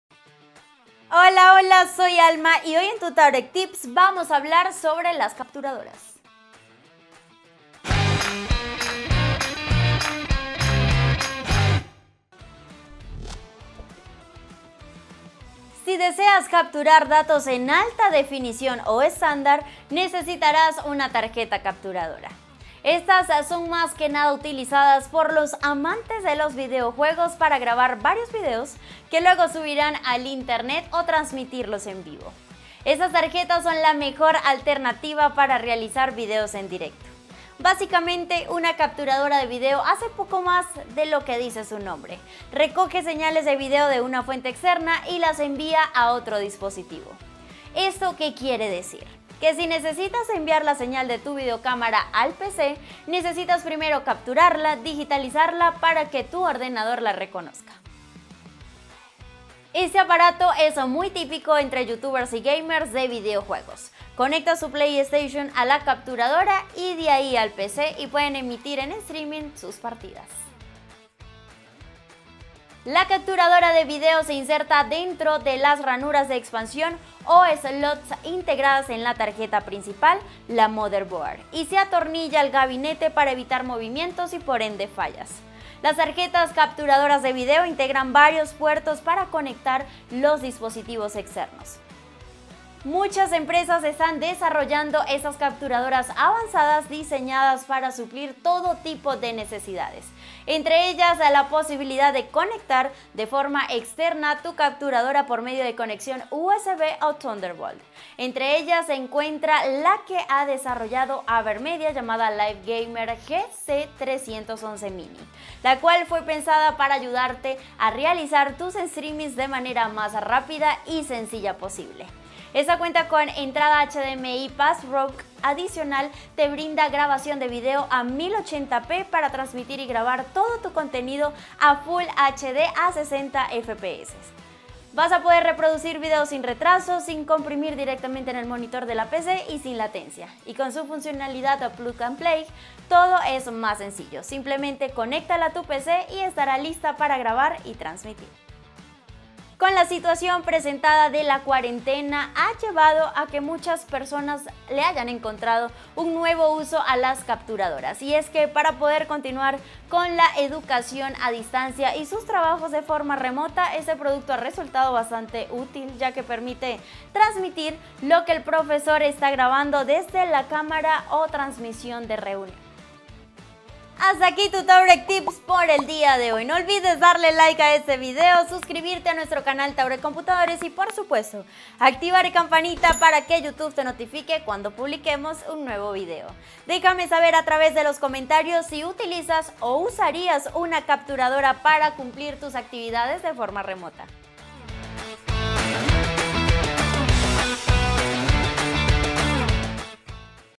Tauret Tips - ¿Qué es una capturadora?
Te explicamos que es una capturadora y cual es su función.